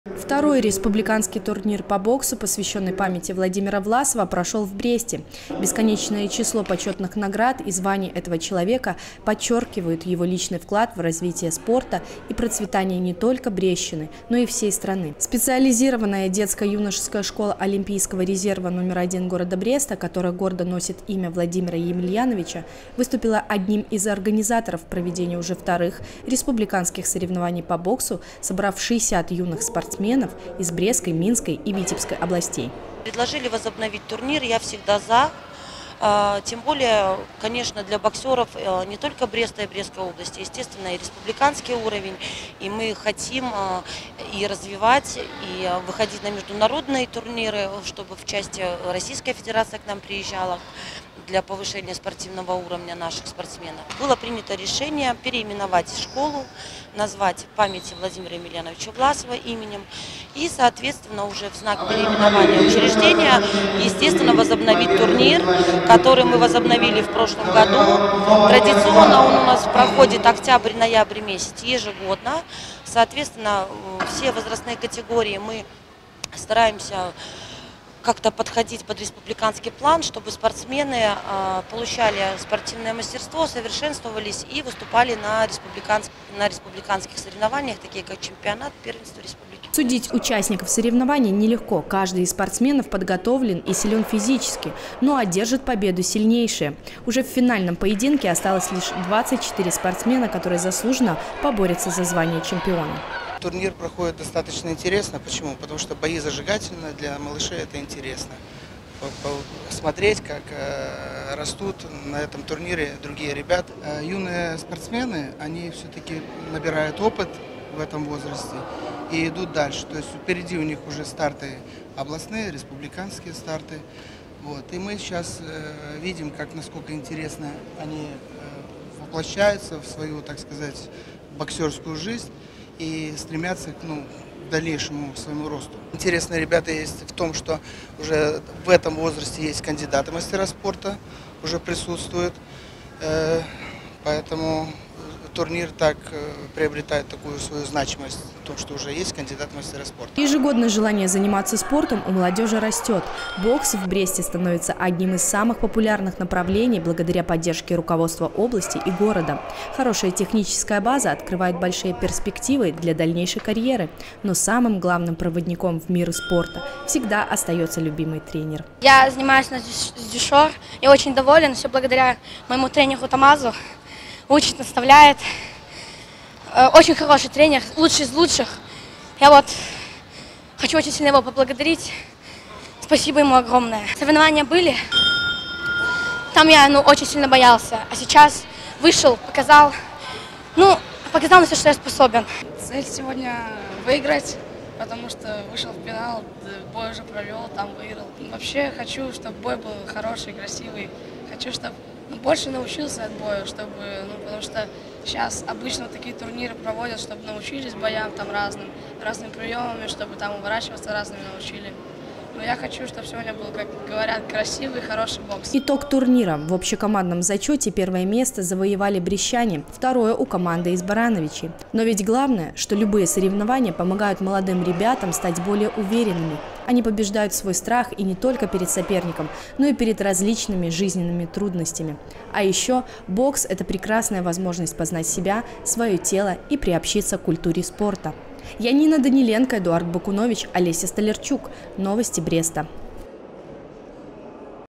2024-11-08 Турнир по боксу, посвящённый памяти В. Власова
Новости спорта. Бокс-один из самых популярных видов спорта. Это не только физическая выносливость, но и сила духа, воспитание моральных и волевых качеств. Он даёт огромный стимул развития характера и личностного роста. А это то, что необходимо подрастающему поколению сегодня. Второй Республиканский турнир по боксу, посвящённый памяти Владимира Власова, прошёл в Бресте. Побороться за звание сильнейшего на ринге спортивного комплекса «Олимп», на этот раз собрались 60 юных спортсменов 2010–2011 годов рождения из Брестской, Минской и Витебской областей. С подробностями Янина Даниленко.

*Наши контакты:*
✅Телефон горячей линии:
54-63-67